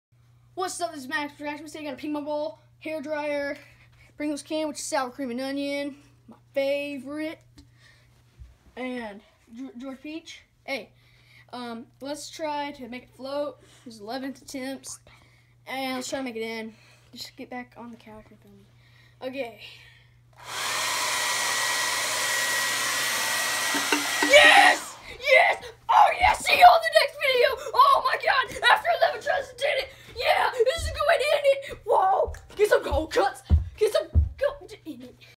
1 hair dryer/ 1 ping pong ball/ 1 Pringles can/ 1 Maximus
I finally made a ping pong trick